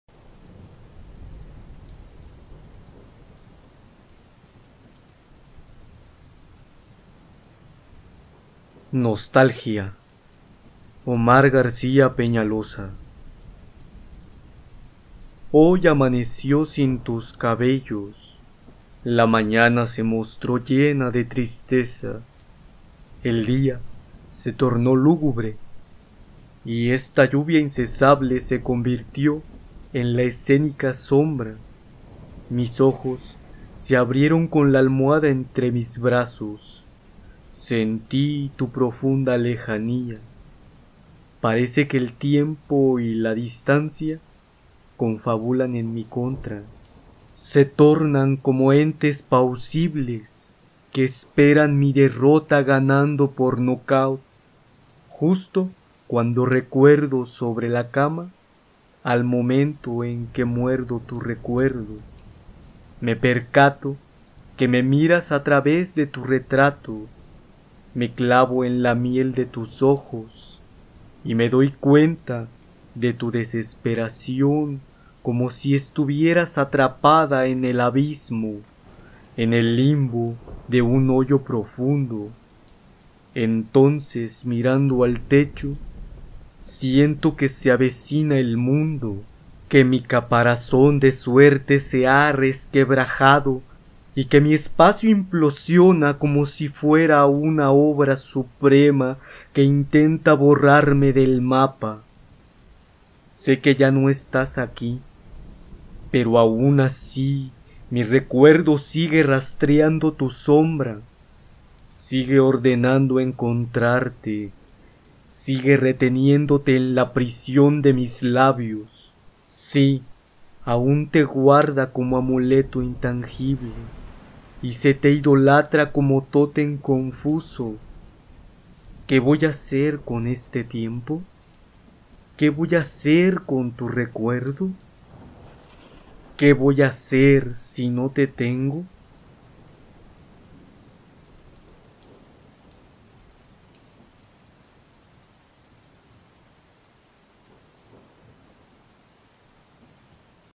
Nostalgia (Omar García Peñaloza)
Título: Nostalgia
Autor: Omar García Peñaloza
Género: Poesía
Fuente: García Peñaloza Omar, Entre sombras, (Chupio, Mpio. de Tacámbaro, Michoacán, México: Taller de Edición Curicaueri, 2019), Págs. 21y 22.

Créditos

Voz:
Iván Canis Corvus